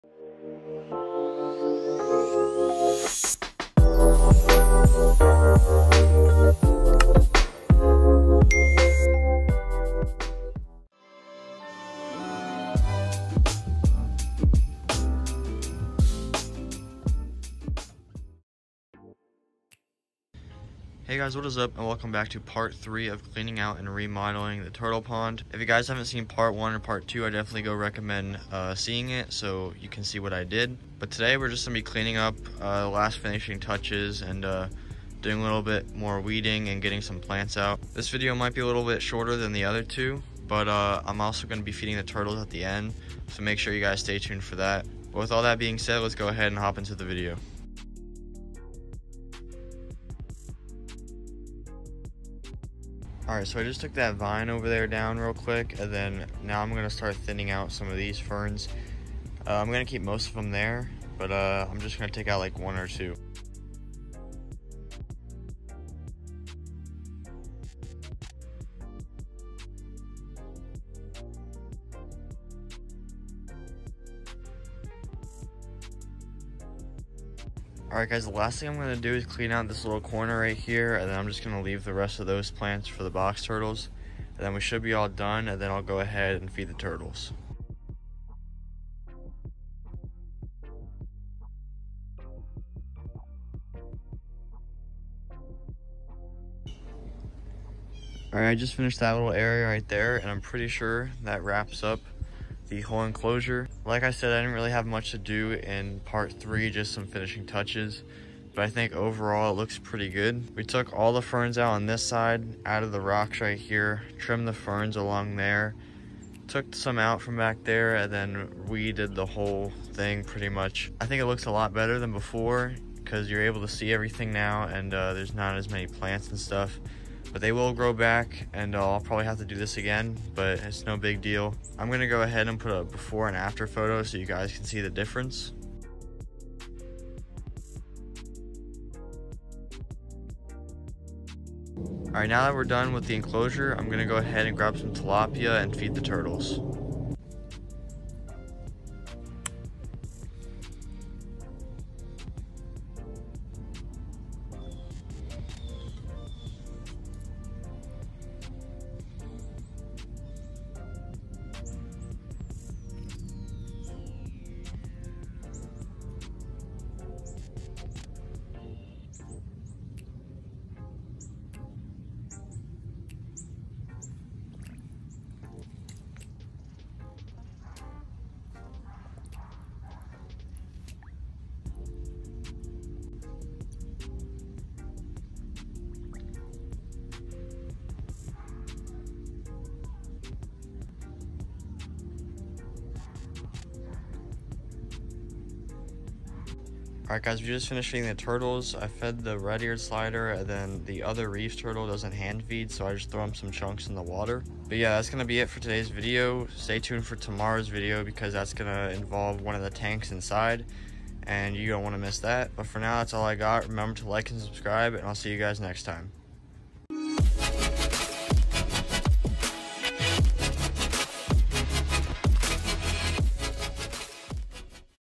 Part 3- Finale. Finishing the turtle pond enclosure!
Hey guys. Welcome to part 3, the finale part of the turtle pond enclosure clean out/ remodel. If you haven’t checked out part 1 and 2 I definitely recommend you do. New video coming tomorrow so stayed tuned. Hope you enjoyed the video. 👍🏻